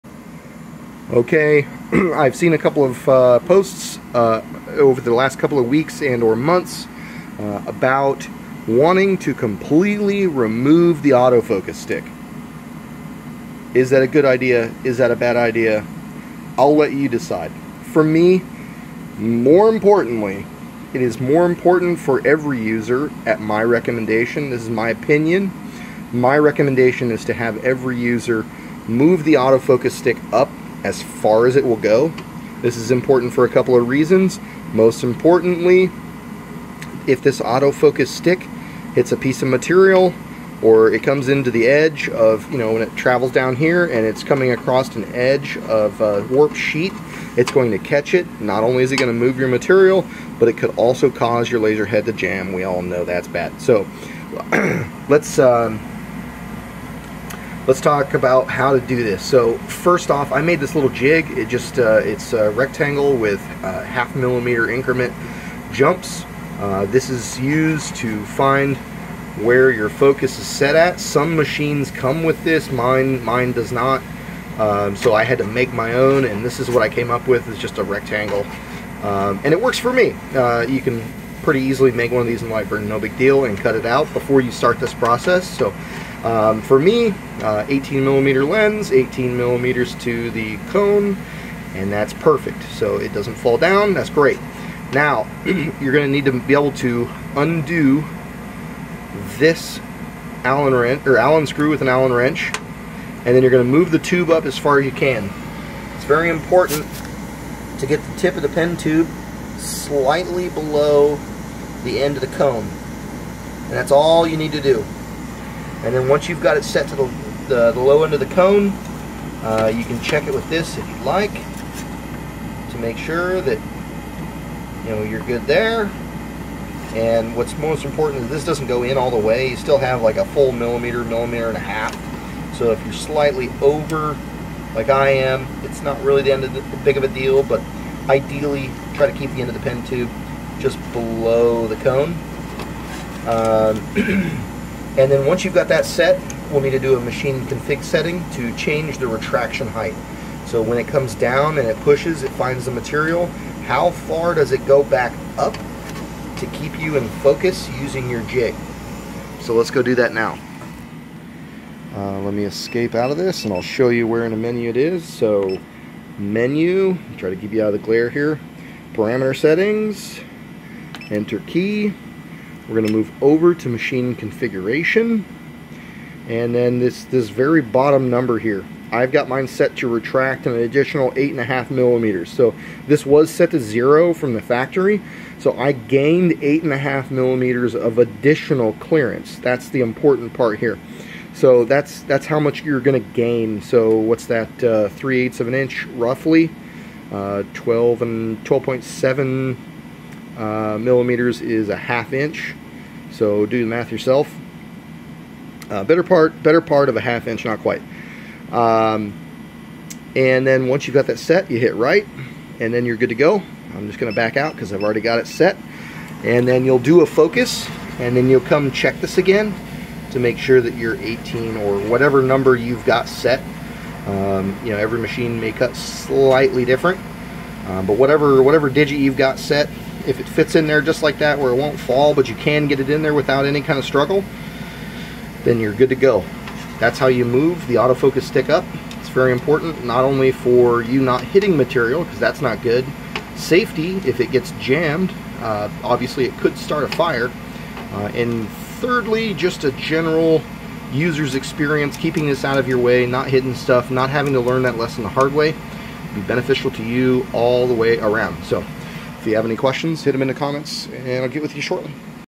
OMTech / Chinese Laser Autofocus Adjustment - Safety/Frustration removal recommendation!
In this video, I go over what it takes to move the autofocus stick and calibrate settings to enhance the user experience as well.
Vivosun 6" fan
OMTech 5200 Chiller
6" Blast Gate
20 Pack 1/8" birch ply